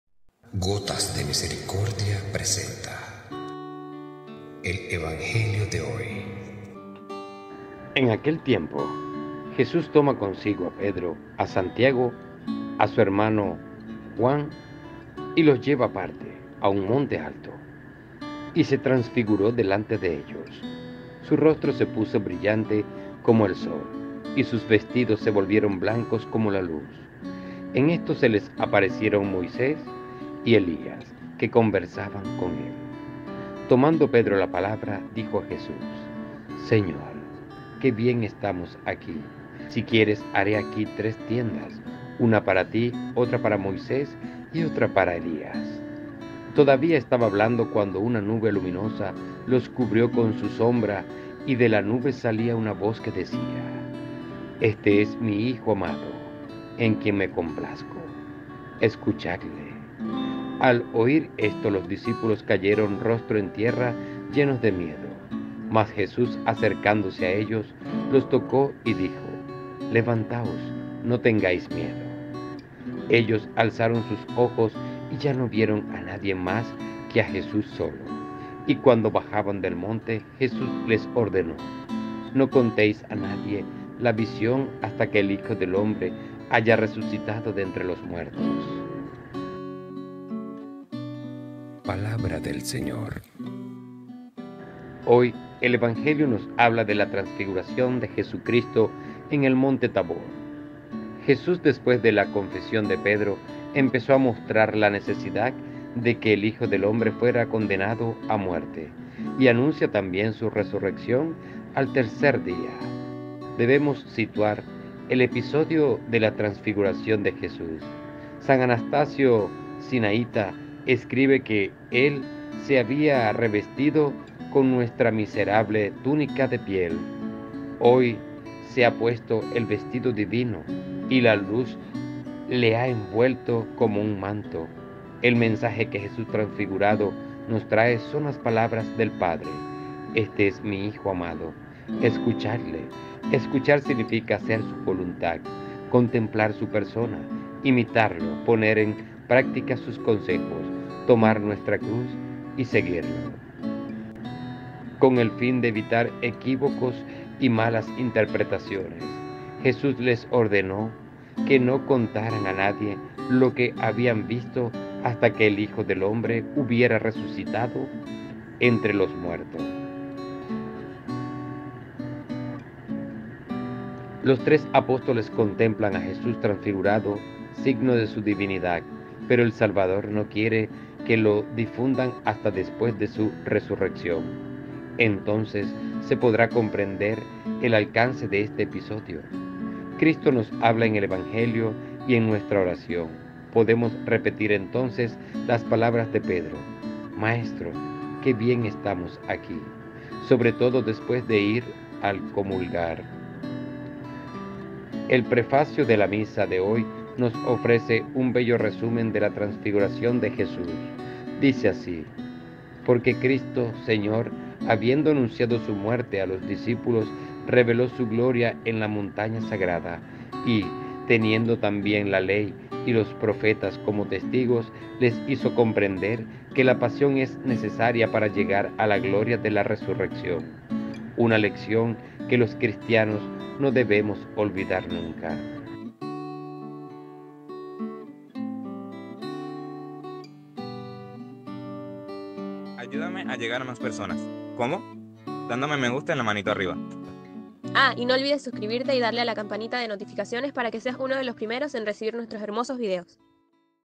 Evangelio de hoy según San Mateo 17,1-9. Domingo 06 de Agosto 2023
"Su rostro se puso brillante como el sol y sus vestidos se volvieron blancos como la luz"
Domingo 6 Agosto. Tiempo Ordinario
La Transfiguración del Señor (A)
«Él se había revestido con nuestra miserable túnica de piel, hoy se ha puesto el vestido divino, y la luz le ha envuelto como un manto».